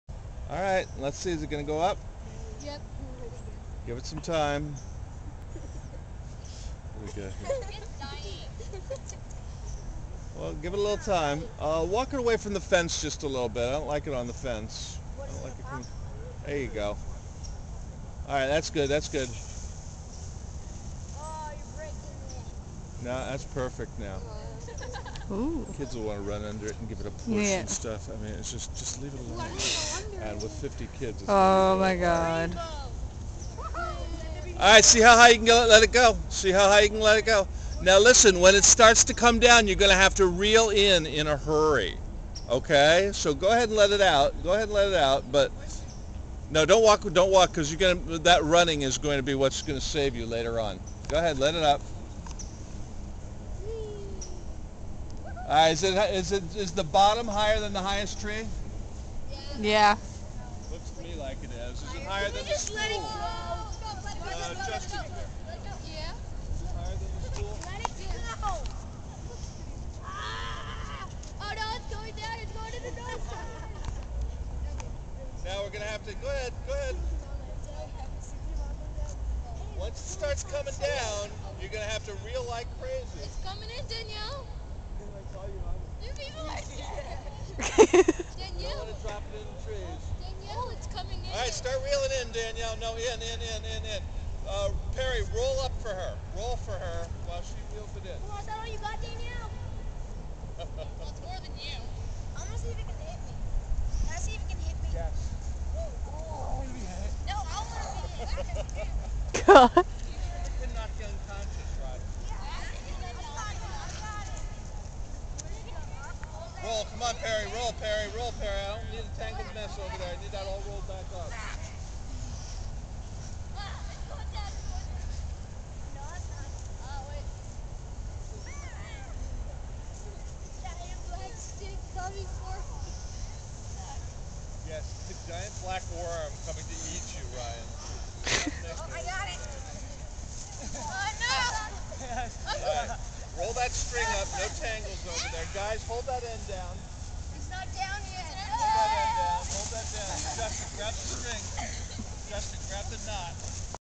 Windbag3.AVI
Finally, a morning when we could fly the windbag and demonstrate buoyancy and Archimedes principle.